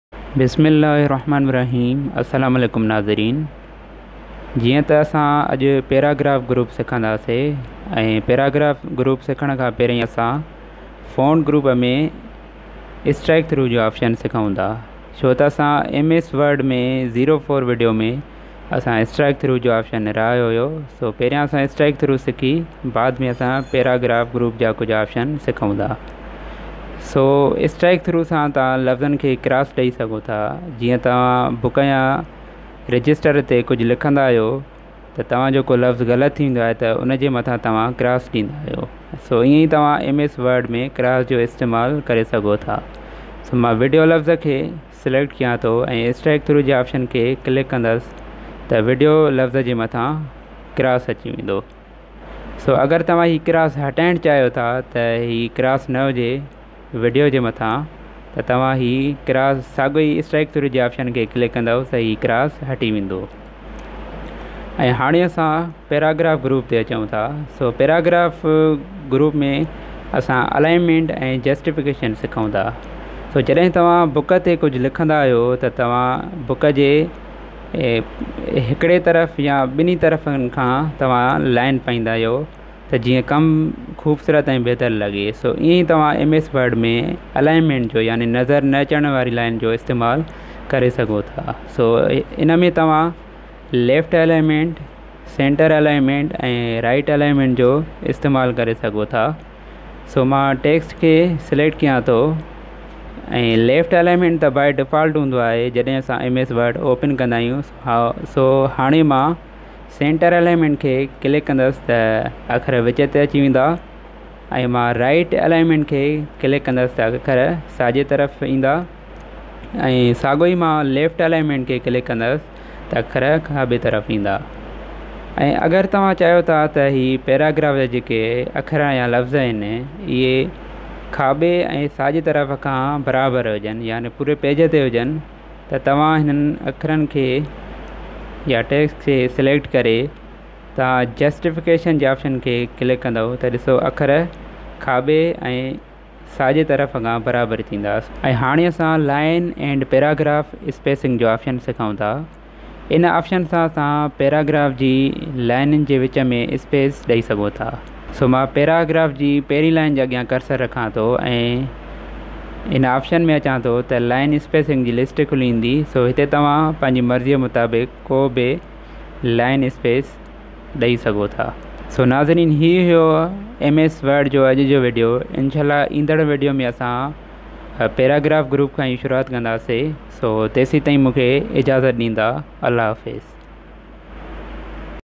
Strike through Align justify line spacing| MS Word in sindhi - MS Word 2016 Tutorial - For beginners
In this video, I have taught one remaining option in font group and some paragraph group options.